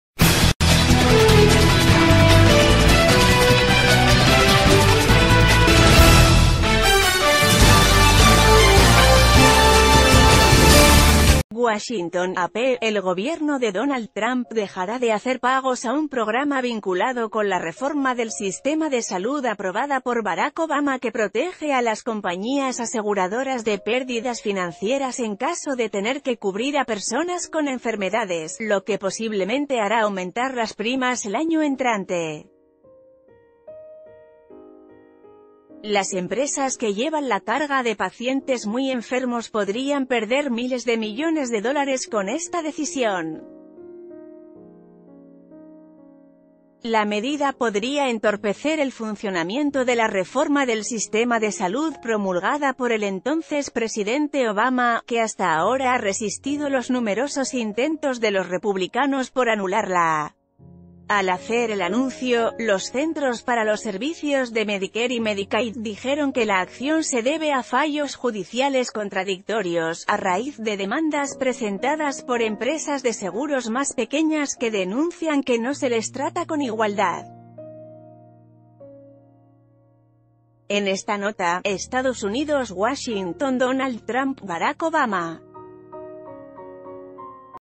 Gobierno de Trump trata de desmantelar reforma de Obama
WASHINGTON (AP) — El gobierno de Donald Trump dejará de hacer pagos a un programa vinculado con la reforma del sistema de salud aprobada por Barack Obama que protege a las compañías aseguradoras de pérdidas financieras en caso de tener que cubrir a personas con enfermedades, lo que posiblemente hará...